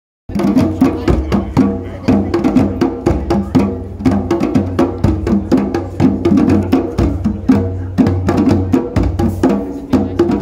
Djembé Trommelgruppe Brücke Schleswig-Holstein Sommerfest 2023 - MUSIC SCHOOL BAUM
Die Djembé Trommelgruppe der Brücke Schleswig-Holstein spielt mit MUSIC SCHOOL BAUM Leiterin Ines Baum auf deren Sommerfest 2023.